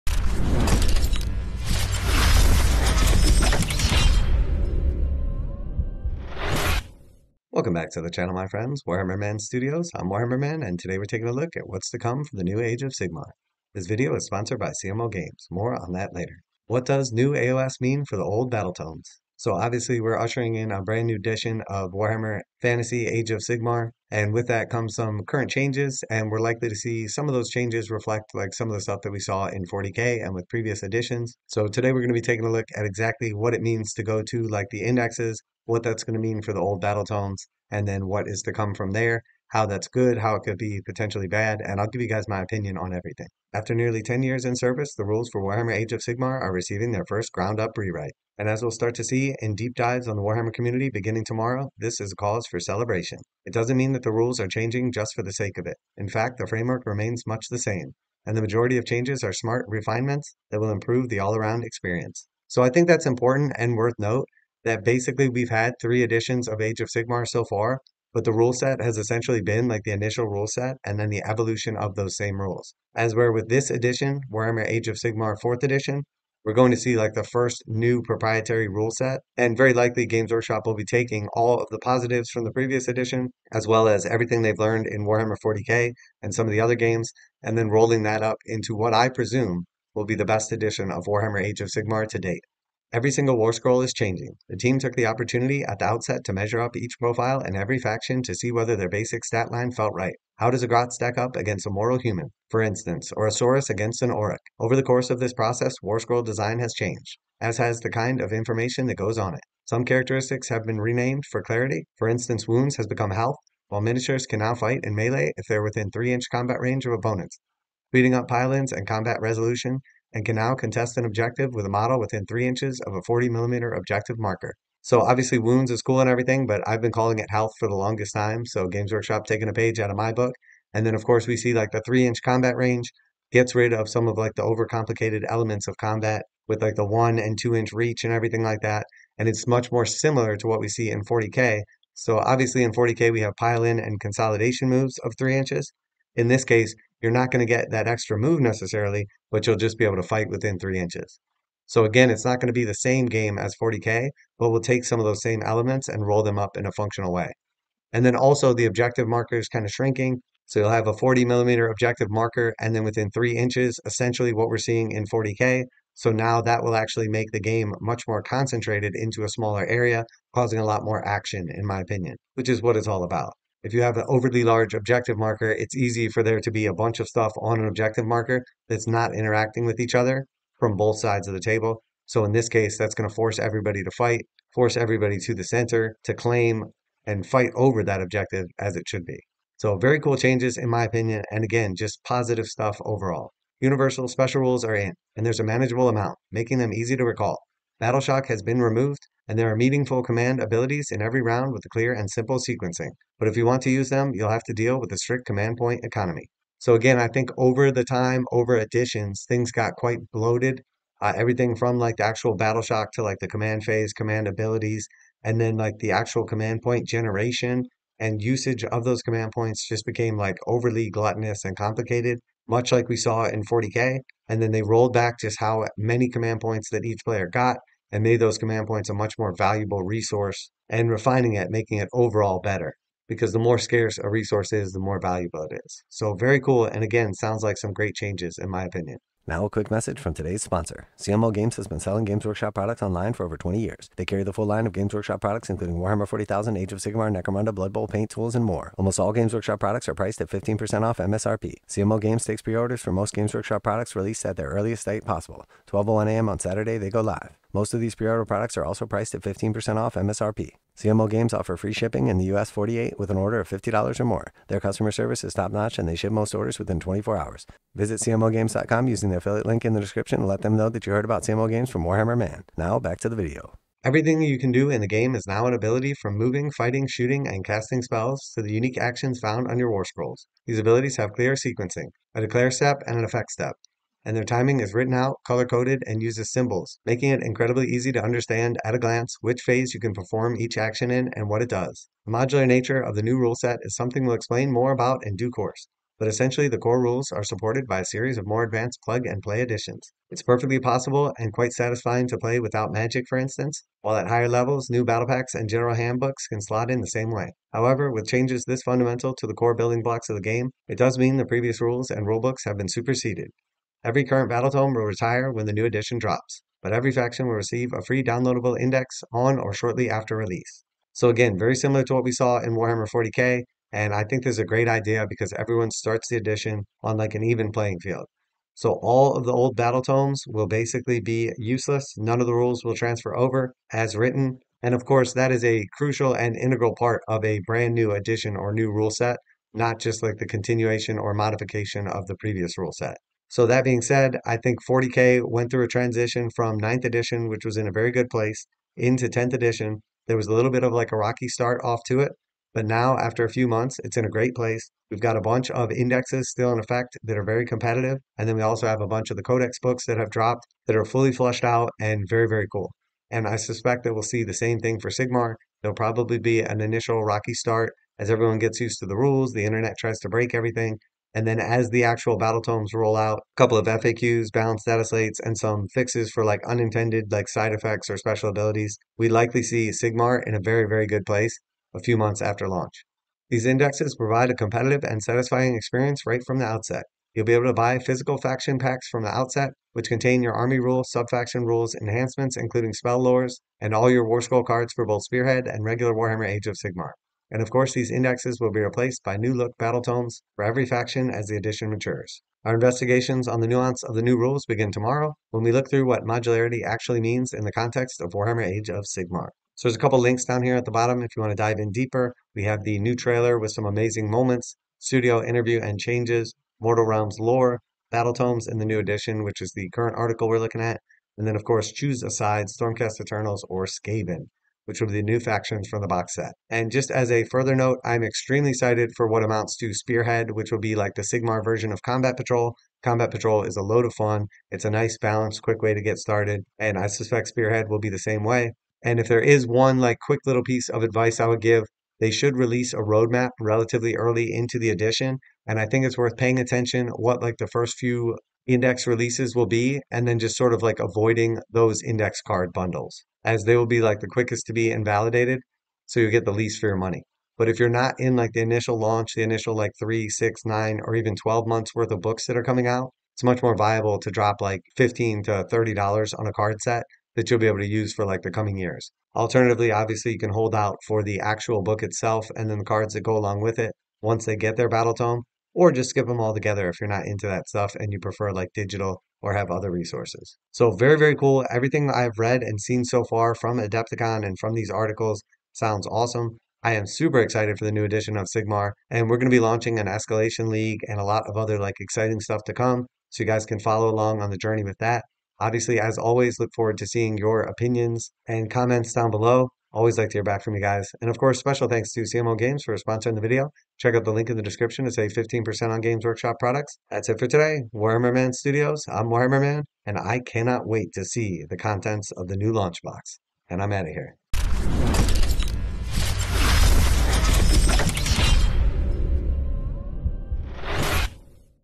NewAOS Games Workshop Just LIT the FUSE... Warhammer Age Sigmar 4th Edition First Look & Breakdown
Games Workshop has officially broken the silence on the new Edition Warhammer AOS 4th ed. Giving the rundown on the new Ground up Edition, Indexes, rules, and how they will effect the game. also more mentions of the Spearhead. Next release and new edition is going to be massive!